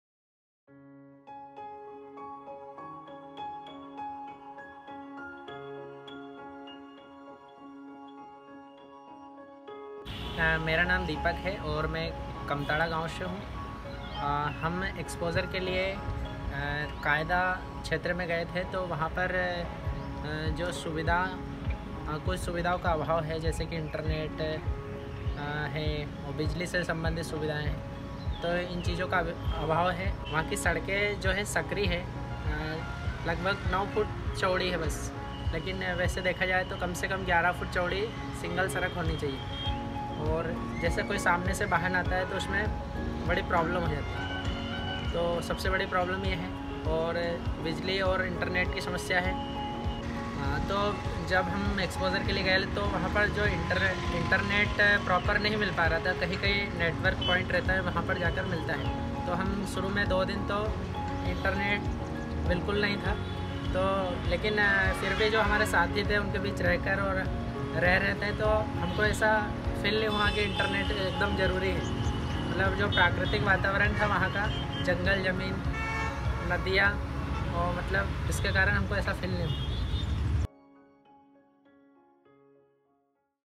5 days in a no internet land! | Group Exposure | Yuvalaya | Paul Hamlyn Foundation
Youth consist more than 50% of internet users in India and they spend at least 2 hours a day.
Life in a no internet land is a reality even now and worrisome for most of us but Deepak found new friends and culture.